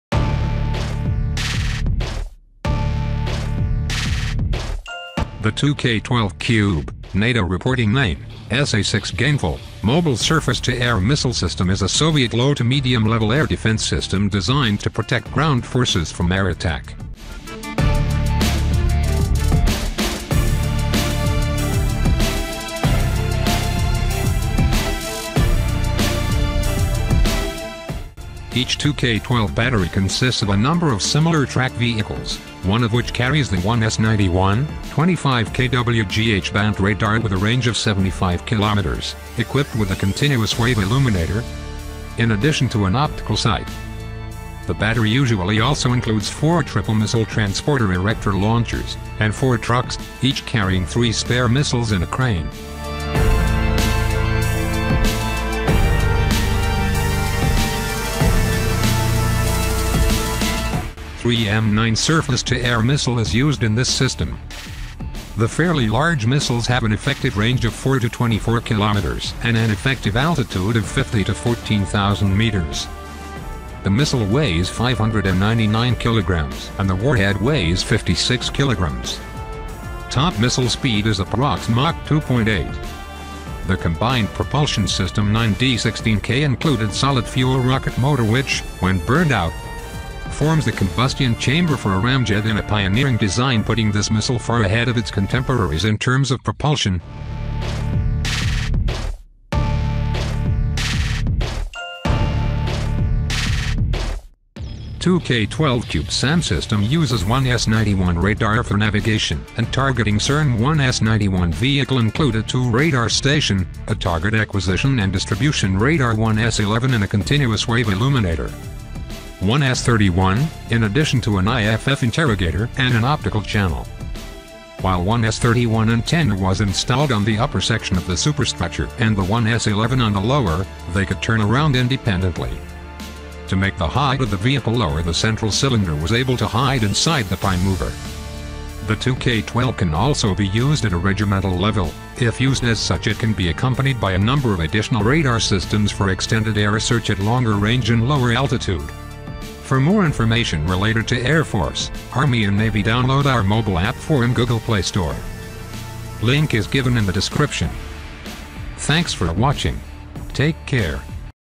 2k12 Kub Surface to Air Missile | Global Powers
The 2K12 "Kub" (English: cube) (NATO reporting name: SA-6 "Gainful") mobile surface-to-air missile system is a Soviet low to medium-level air defense system designed to protect ground forces from air attack.